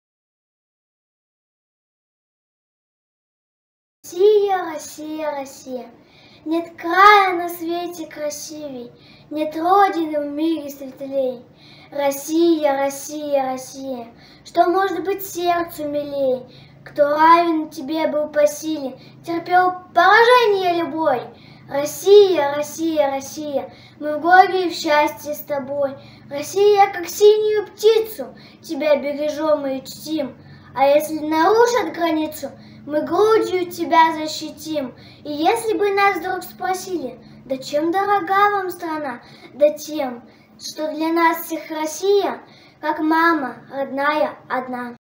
Гурьянова Даша читает стихотворение «Россия, Россия, Россия», автор Владимир Гудимов (с. Узкий Луг)
Районный патриотический конкурс чтецов «Мы говорим стихами о России»